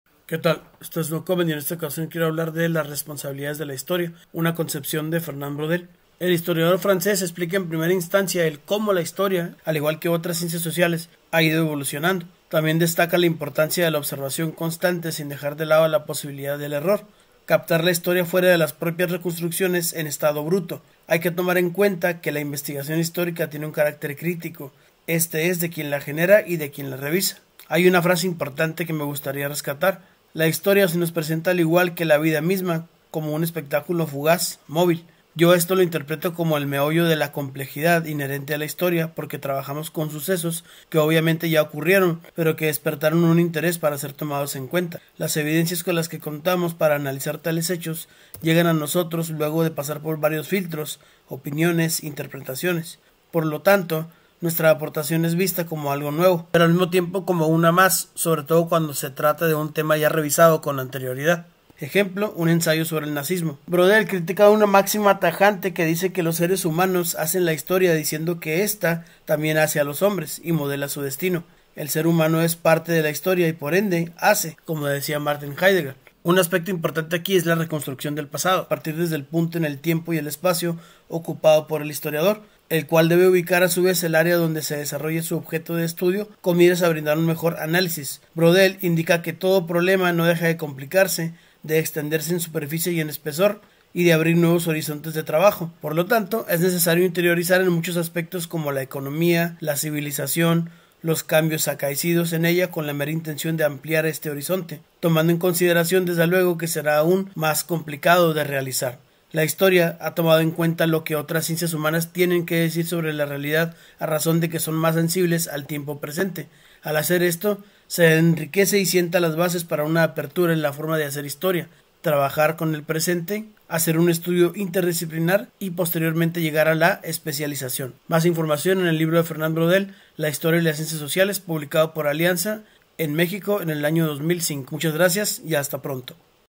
Teoría de la historia: Las responsabilidades de la historia
Fernand Braudel apuntó que la historia debe cumplir con ciertas responsabilidades. Aquí los detalles. Cheers.